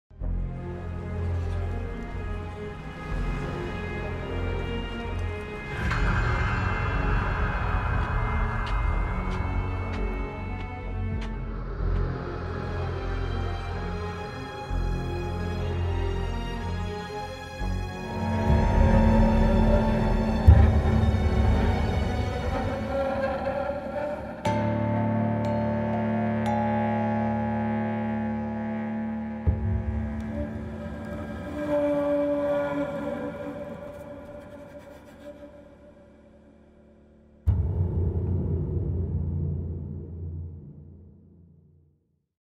Lejligheden - bogtrailer
Lejligheden er en mesterlig psykologisk ægteskabsthriller. Læs mere om bogen her